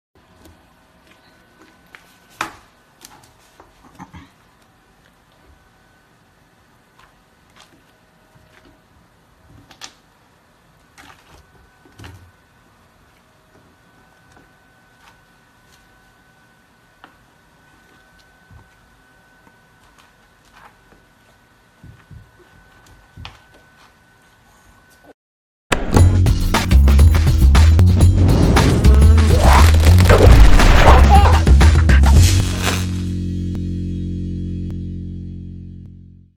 Samoyed puppy extremely intrigued by ice cube
This adorable fluffy Samoyed puppy has his first encounter with an ice cube and it's just too cute. Must see!